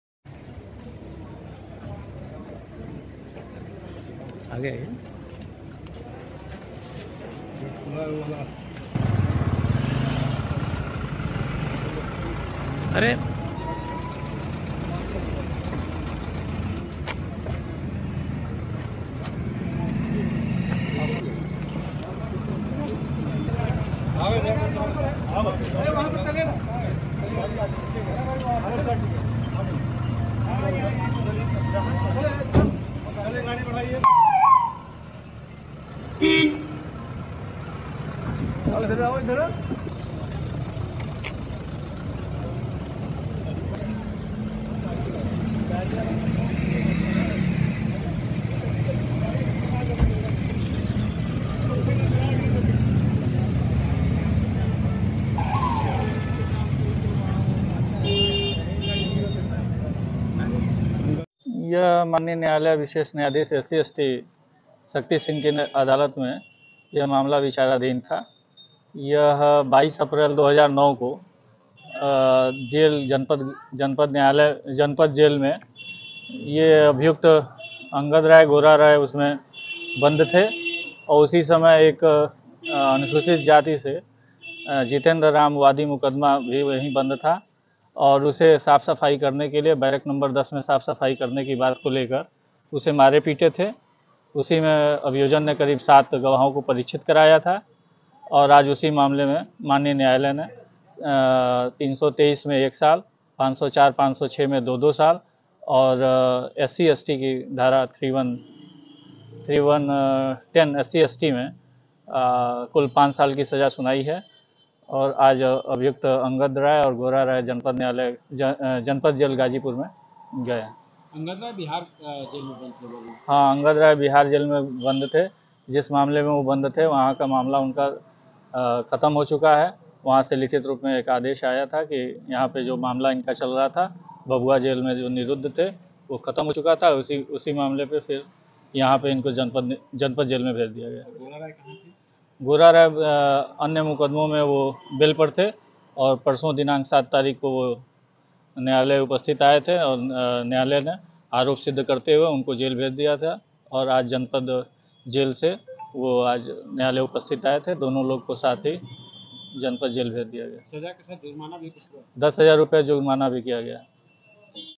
Mukhtar Ansari के करीबी शूटर Angad Rai को सज़ा, 15 साल पुराने मामले में फैसला | Ghazipur | UP Crime
मुख्तार अंसारी के करीबी शूटर अंगद राय और गोरा राय को कोर्ट ने सजा सुनायी है।गाजीपुर की एससी/एसटी कोर्ट ने दोनों को 5-5 साल की सजा दी है।